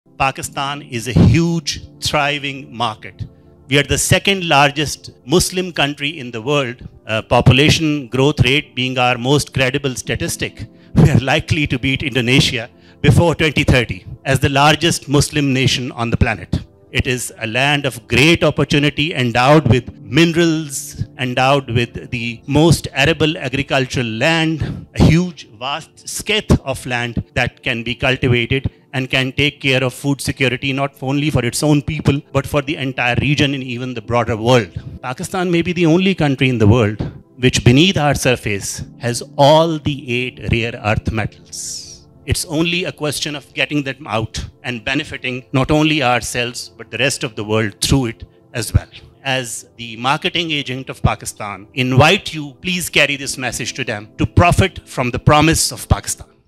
Pakistan Stability on Natural Resources, Economic Situations | Rizwan Saeed | Pakistan at Davos 2024
A passionate description of Pakistan as a thriving market as the second largest Muslim country. Rizwan Saeed talks about compelling statistics and the vast opportunities the nation has.

Speaker: Rizwan Saeed - Additional Foreign Secretary MOFA
Event: Inspiring Lunch Event at Pakistan at Davos 2024